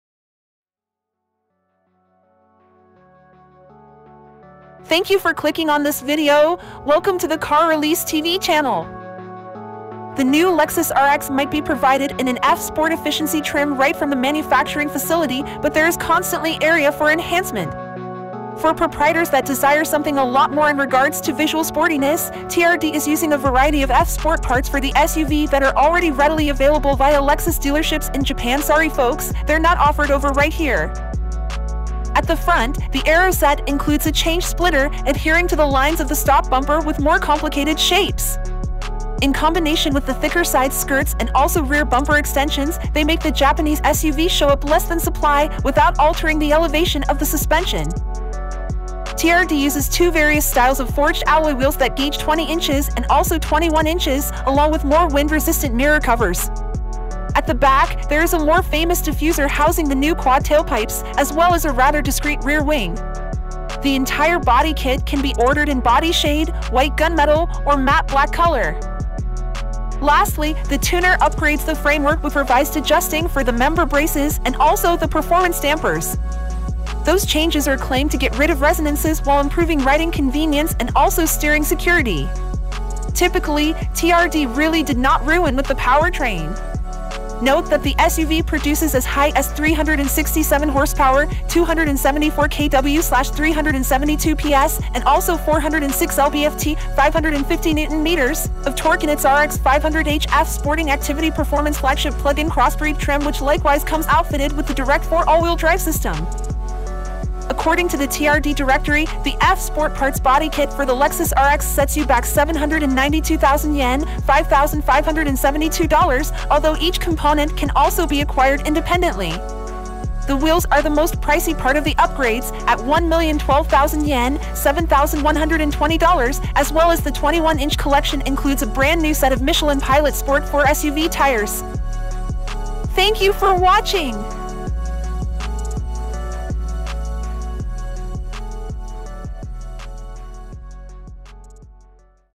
[BREAKING NEWS] 2023 Lexus RX 500h F Sport Model | Sportier Looks And Chassis Tuning Thanks To TRD!
2023 Lexus RX 500h F Sport - The new Lexus RX might be provided in an F Sport Efficiency trim right from the manufacturing facility, but there is constantly area for enhancement. For proprietors that desire something a lot more in regards to visual sportiness, TRD is using a variety of F Sport parts for the SUV that are already readily available via Lexus dealerships in Japan-- sorry folks, they're not offered over right here.